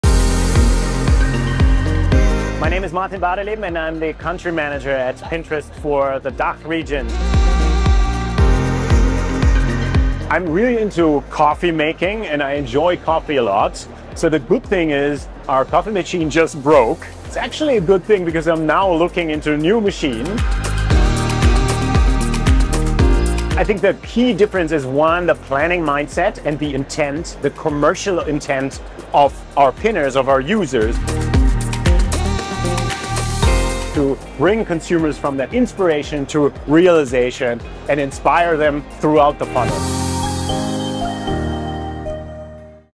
What differentiates Pinterest from other platforms
If you want to connect with consumers during the shopping journey, meet them where they’re planning their next purchase: on Pinterest. Martin Bardeleben, our DACH Country Manager, explains what differentiates Pinterest from other platforms and what that means for advertisers.